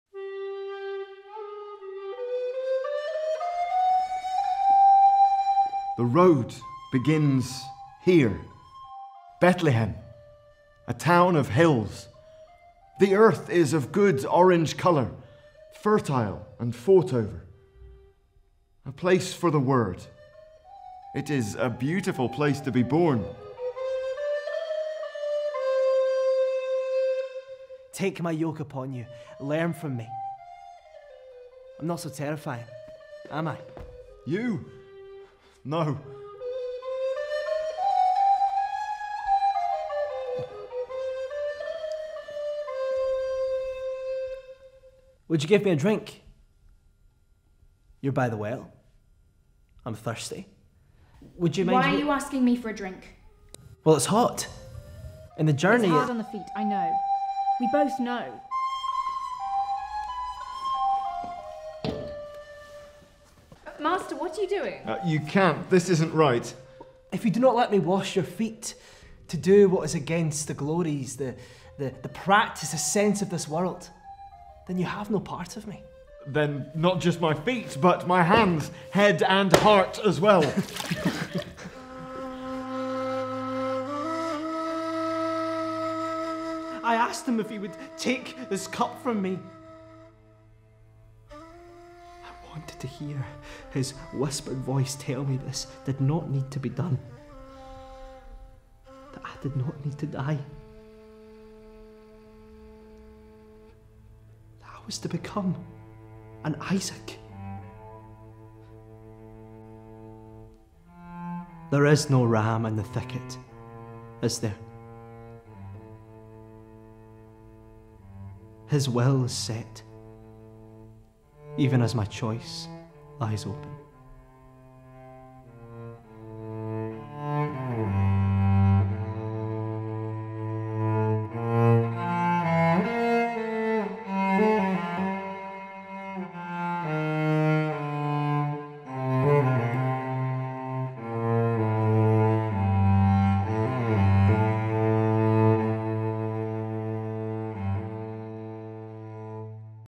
The Narrow Road - Passion Play 2024
The Narrow Road by Paul Birch, our acclaimed passion play, touring the UK throughout the season of Lent and Easter in 2024.

For more information and to book a performance, please go